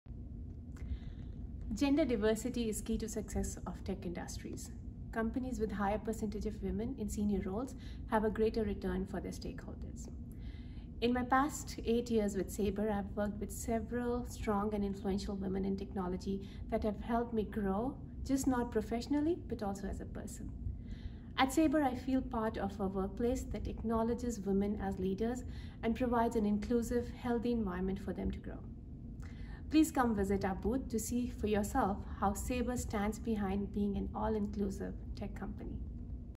Charu Srivastava's message to women technologists
Charu Srivastava is the Director - Program Management, Data & Analytics at Sabre Corporation. She speaks about the inclusive work culture at Sabre and what helped her grow professionally and personally. Visit our stall (B17) at the 2019 Grace Hopper Annual Conference to meet our team!